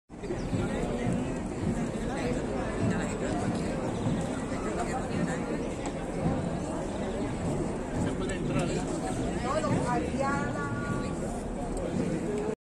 [4K] ALFOMBRA 100 AÑOS | IGLESIA DE LA VICTORIA | SANTA MISIÓN DE LA VIRGEN DEL CARMEN | JEREZ 2024
⛪ IGLESIA DE LA VICTORIA "JEREZ DE LA FRONTERA"

🎥 POLILLA_COFRADE